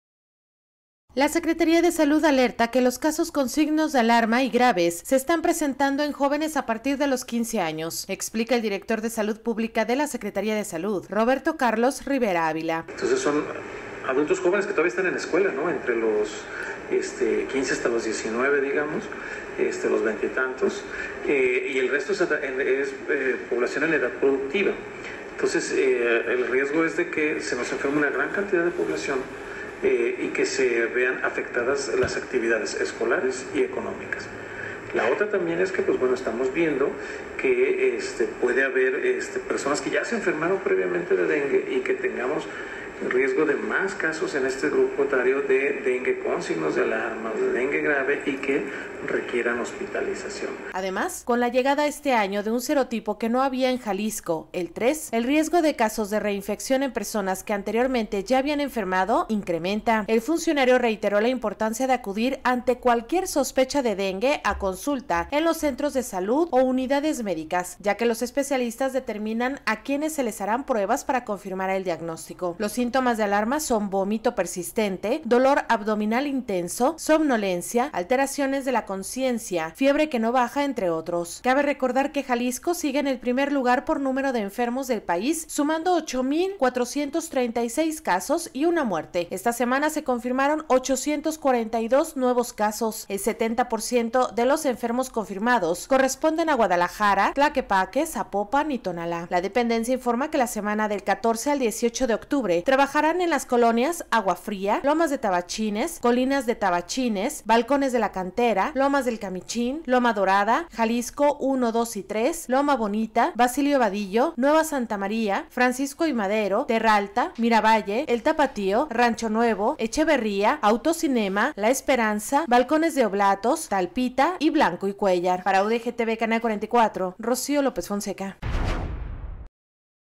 Adolescentes y jóvenes principales afectados por dengue con signos de alarma y grave
La Secretaría de Salud alerta que los casos con signo de alarma y graves se están presentando en jóvenes a partir de los 15 años, explica el director de Salud Pública de la Secretaría de Salud, Roberto Carlos Rivera Ávila: